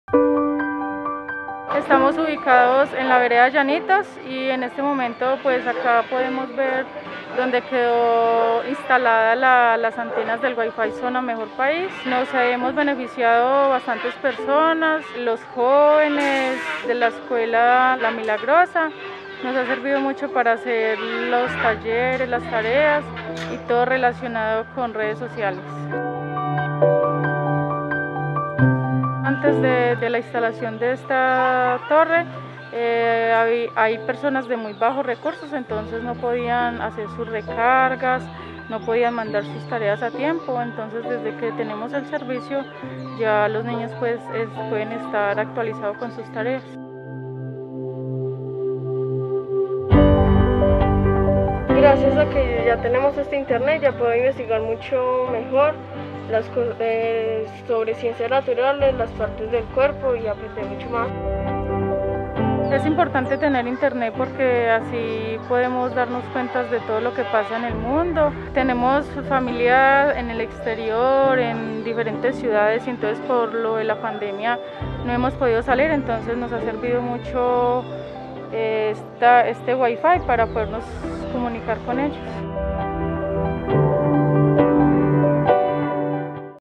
'El internet nos ha dado la oportunidad de comunicarnos con la familia en el exterior'
Dageny y su familia hacen parte del grupo de ciudadanos beneficiados con la zona digital de la vereda Llanitos en el departamento de Caldas. Ella explica que la conectividad brindada por este punto les ha permitido avanzar en las tareas del día a día y les ha ayudado a comunicarse con sus familiares en otros lugares del mundo.